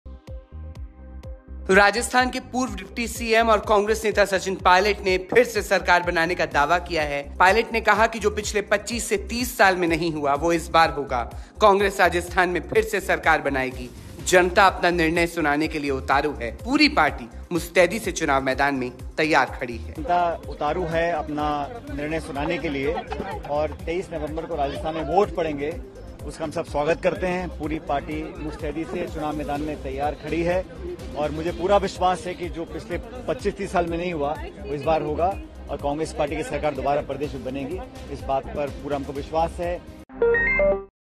Rajasthan Election: पायलट का दावा- जो 30 साल में नहीं हुआ वो इस बार होगा
राजस्थान के पूर्व डिप्टी सीएम और कांग्रेस नेता सचिन पायलट ने फिर से सरकार बनाने का दावा किया है. पायलट ने कहा कि जो पिछले 25-30 साल में नहीं हुआ वो इस बार होगा और कांग्रेस राजस्थान में फिर से सरकार बनाएगी. जनता अपना निर्णय सुनाने के लिए उतारू है. पूरी पार्टी मुस्तैदी से चुनाव मैदान में तैयार खड़ी है.